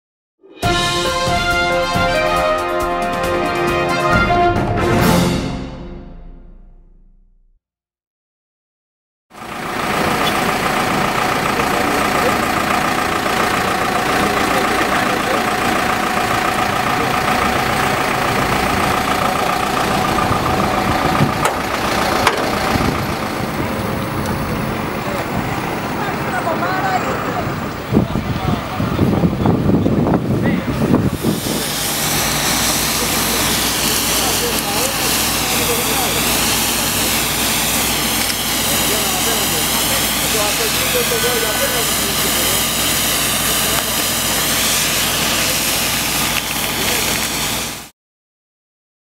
ilialive.gr - Καραμπόλα στην Αγ. Γεωργίου
ilialive.gr - Καραμπόλα στην Αγ. Γεωργίου - 28 Ιουλ 2012 - Καραμπόλα με την εμπλοκή τριών αυτοκινήτων συνέβη το απόγευμα του Σαββάτου 28/7 στην Ε.Ο. Πατρών - Πύργου, στο ύψος της διασταύρωσης Αγ. Γεωργίου.